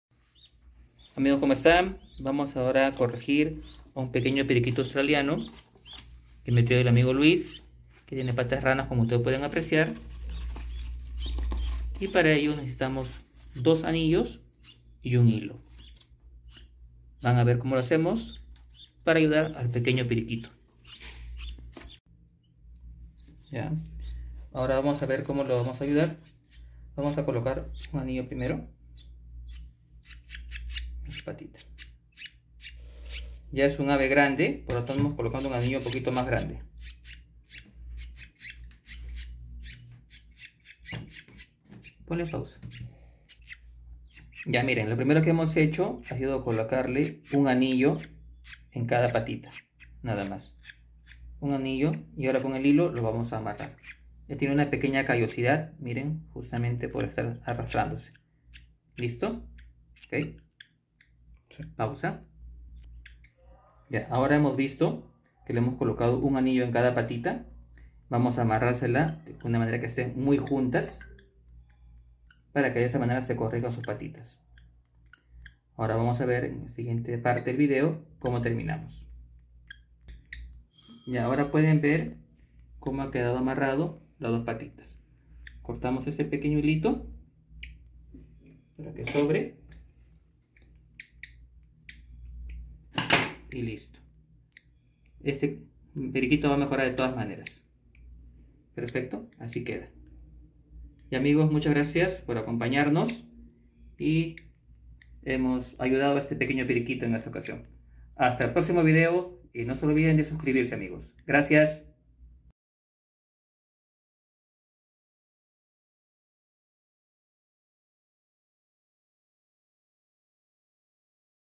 Corrigiendo las patas de rana en un periquito australiano
Amigos en esta ocasión con el amigo Luis Fernández del canal amigo LUCHO ANIMALS, quien trajo un periquito australiano que presentaba patas de rana, usamos la técnica de colocar dos anillos, uno en cada pata y amarrarlos, esperemos que todo le vaya bien, hasta el siguiente video, saludos, acá le dejo el enlace del canal amigo